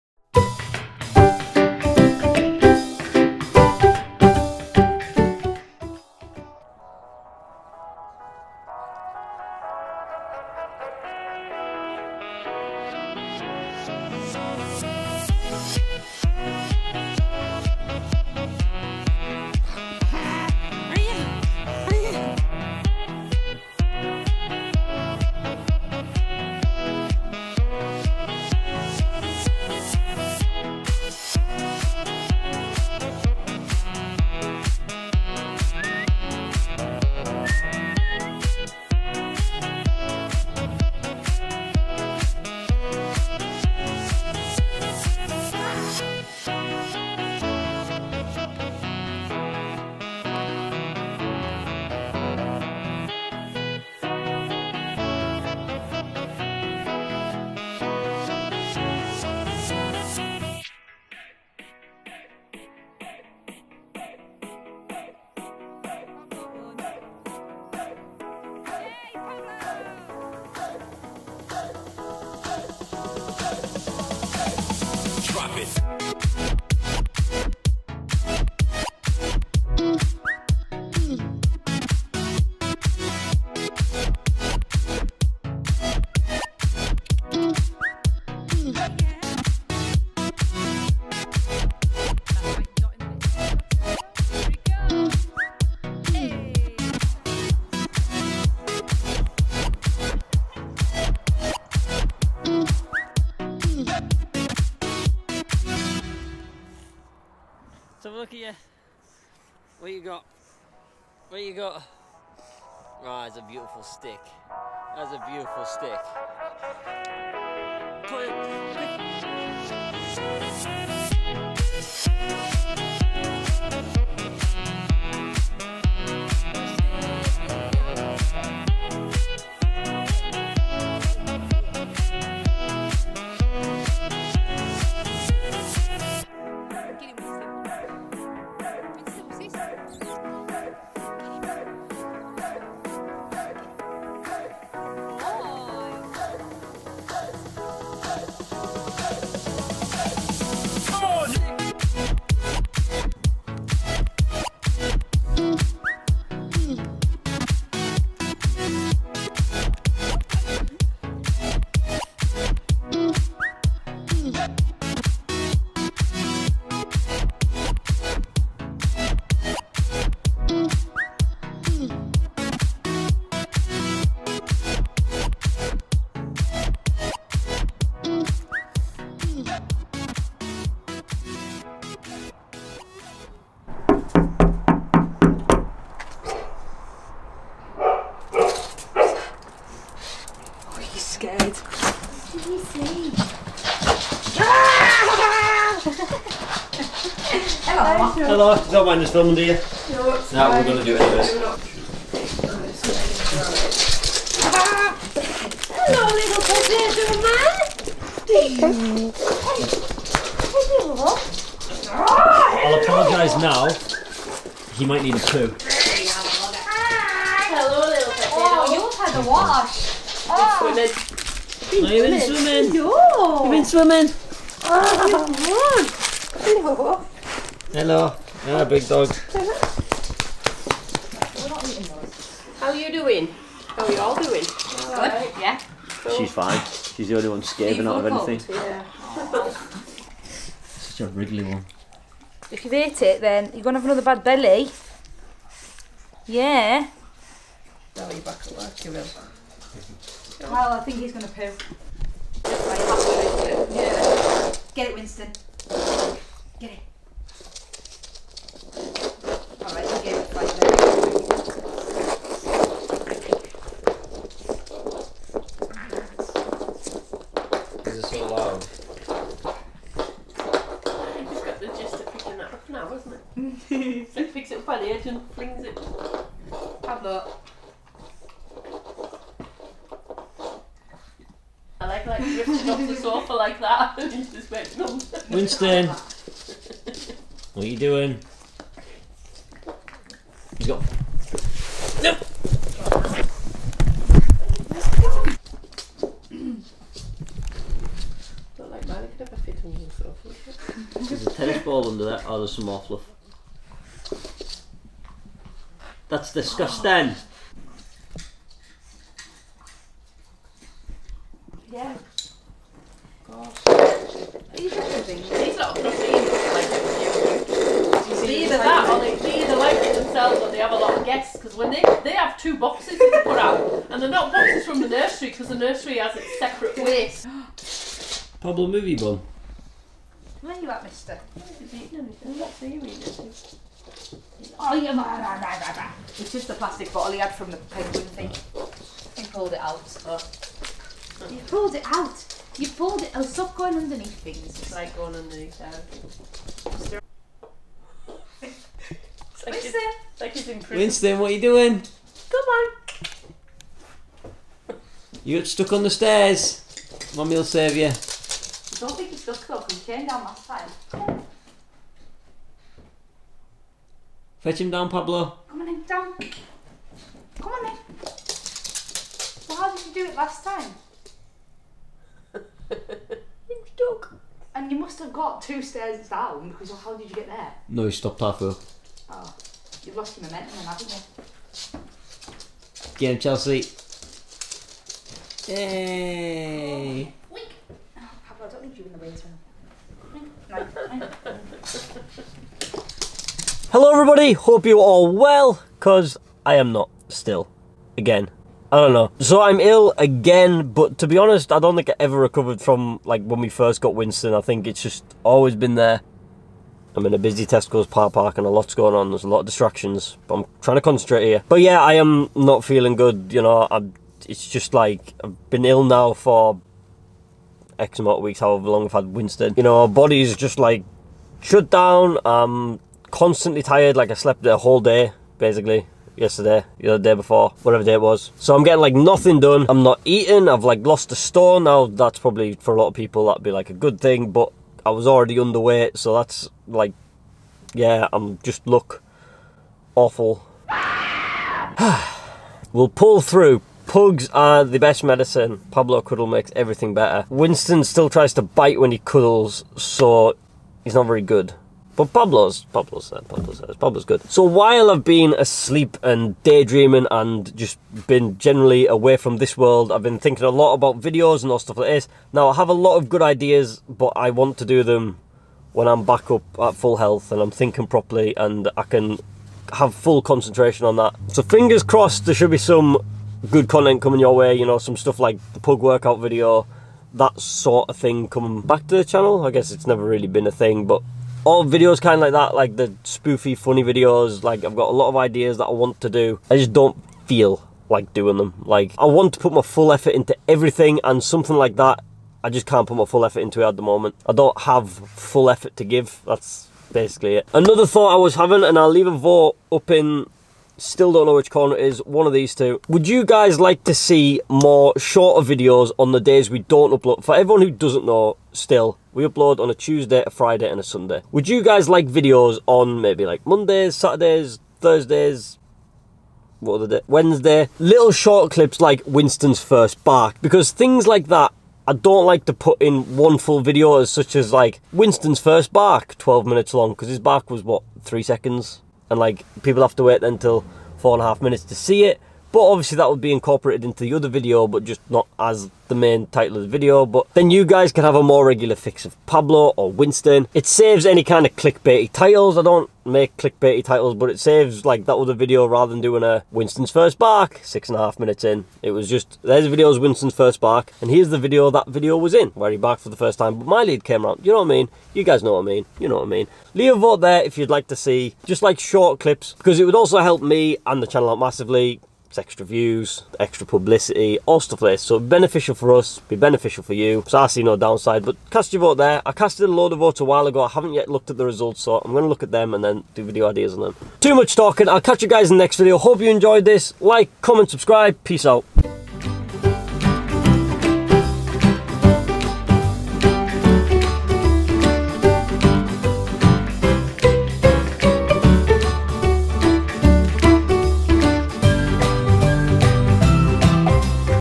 Running Around and Visiting Friends!
Join us on a regular event of talking the boys for a walk and visiting my mum and Mylie! Winston is getting very confident and is exploring EVERY inch of my mum's house ..

Licence;